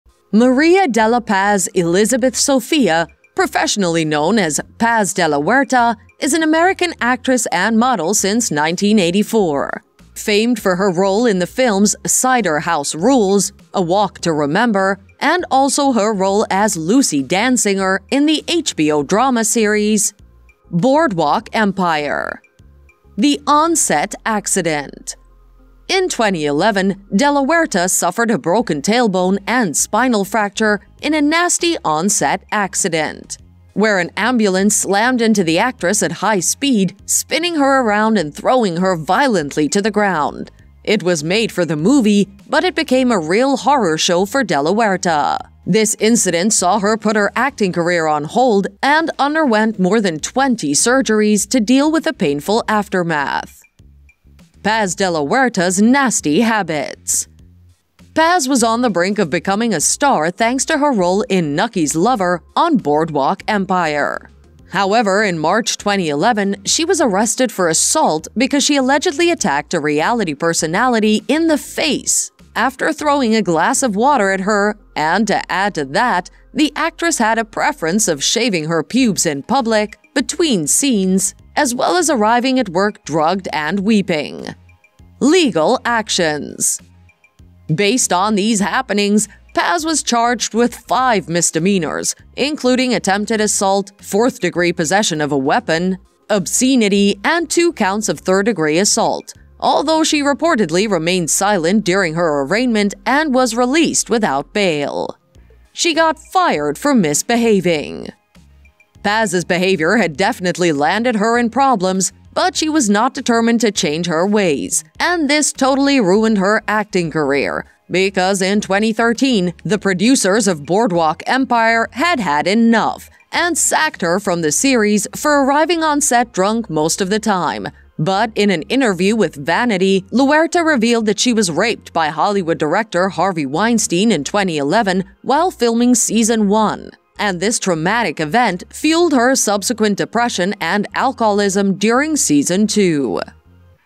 Why Paz De La Huerta’s Career Died So Quickly
Maria de la paz Elizabeth Sofia , professionally known as Paz de la Huerta is an American actress and model since 1984 famed for her  roles in the films ‘Cider House Rules, A walk to remember, and also her role as  Lucy Danziger in The HBO drama series Board walk Empire.